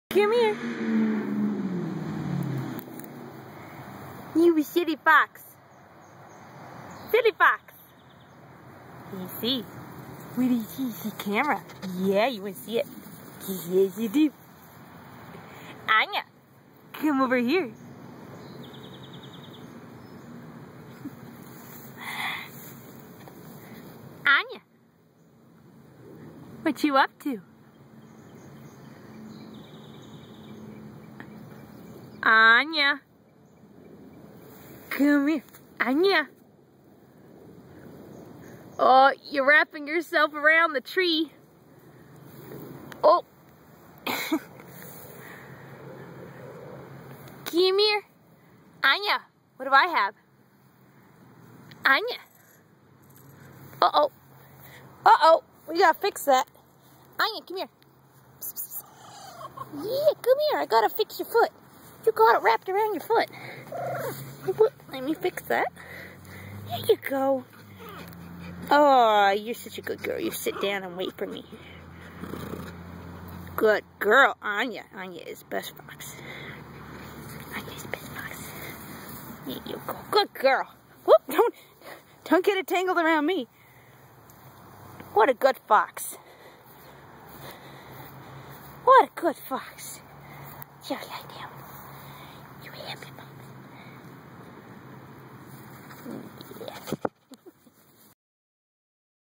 Anya on a lead
Anya tends to wander a bit, so Kay hooks her harness to a lead so she can roam without getting lost. To learn more about domestic foxes, go to domesticfox.com